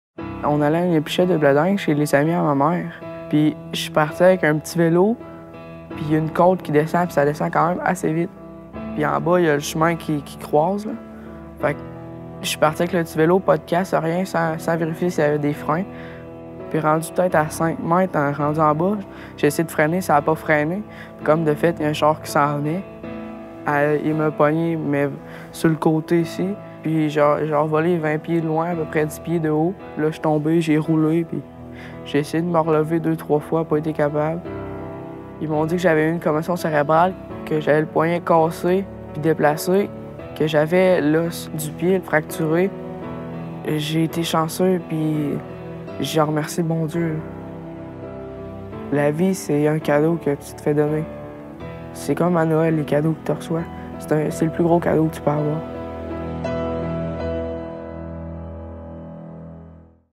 Témoignage de Maxime Bussière
Maxime Bussière a été heurté par un véhicule alors qu'il était à vélo.